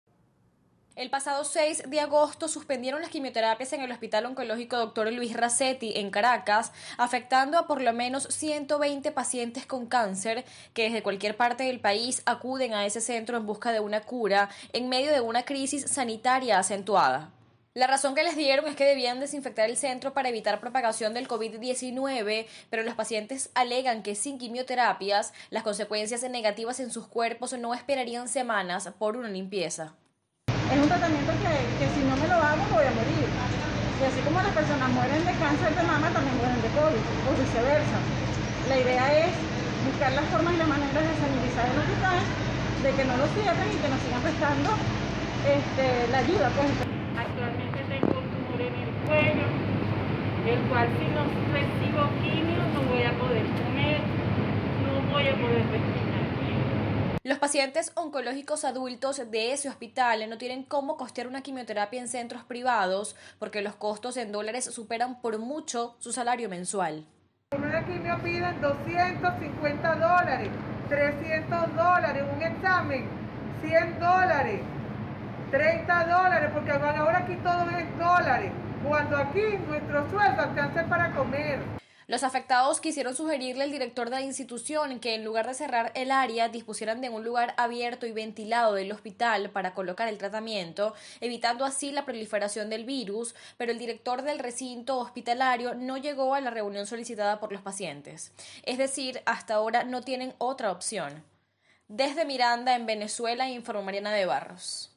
Pacientes oncológicos denuncian suspensión de quimioterapias en Venezuela
Pacientes oncológicos denuncian que en el Hospital Oncológico Doctor Luis Razetti, en Caracas, suspendieran las quimioterapias hasta septiembre, por una supuesta desinfección que le harán al centro, para evitar contagios por COVID-19.